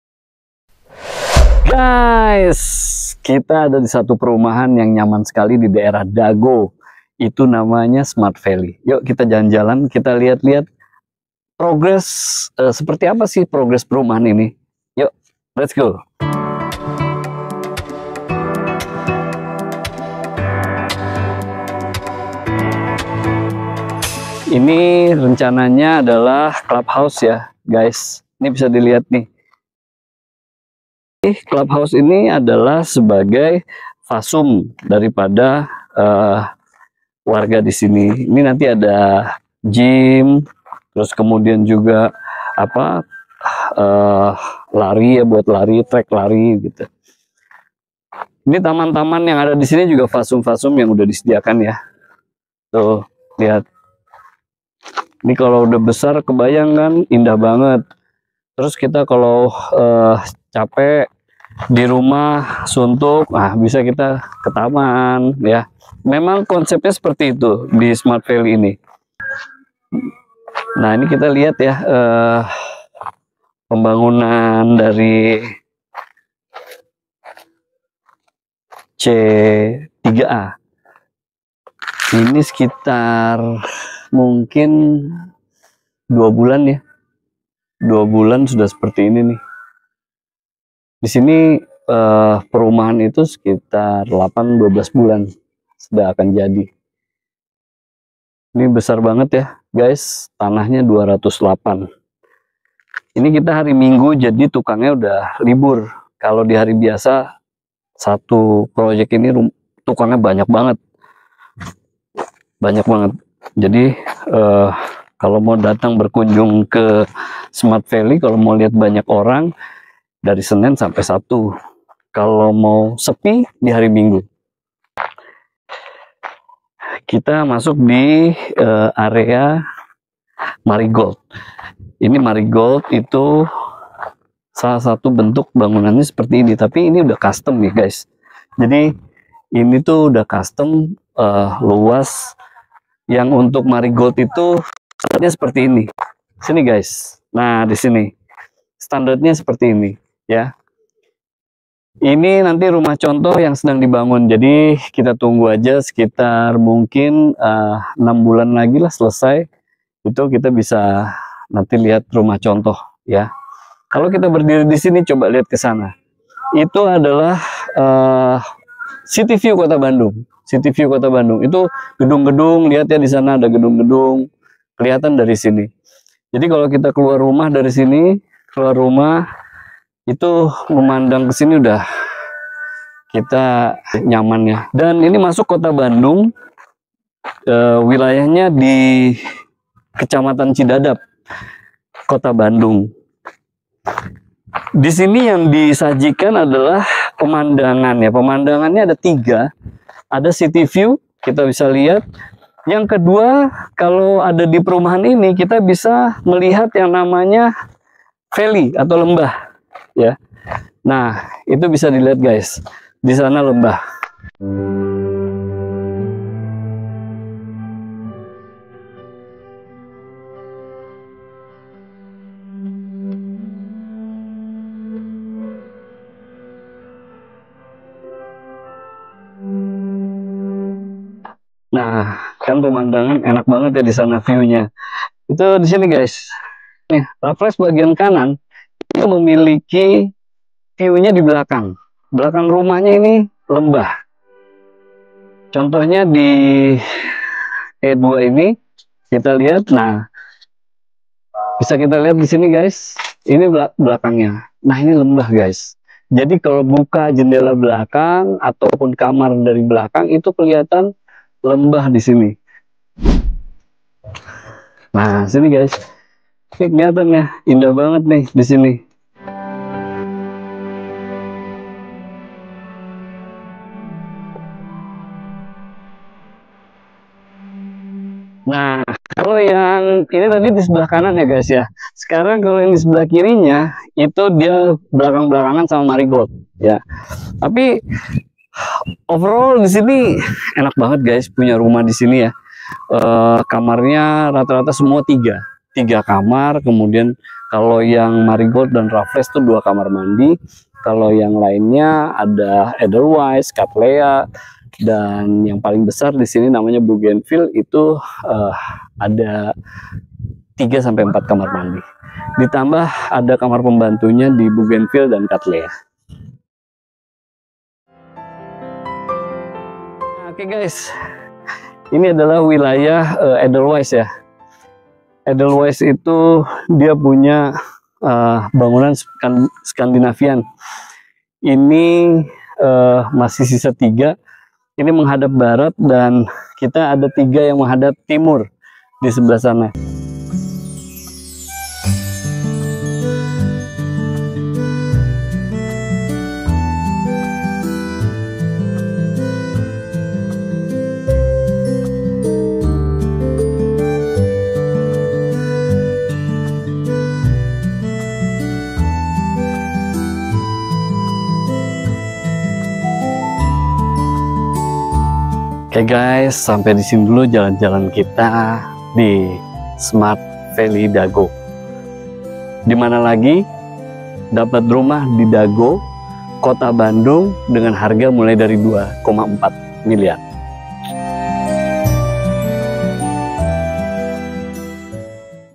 Perumahan Nyaman di Dago Bandung Smart Valley
RUMAH SULTAN
Perumahan Elite di Kawasan Bandung Utara dengan Konsep Mewah Minimalis. Suasana hunian yang asri, nyaman dan sejuk menghadirkan taman tematik bagi penghuninya sebagai tempat beraktivitas outdoor. Smart Valley memiliki lingkungan hijau dan udara sejuk di pegunungan, serta
Dimana lagi kalau bukan di SMART VALLEY DAGO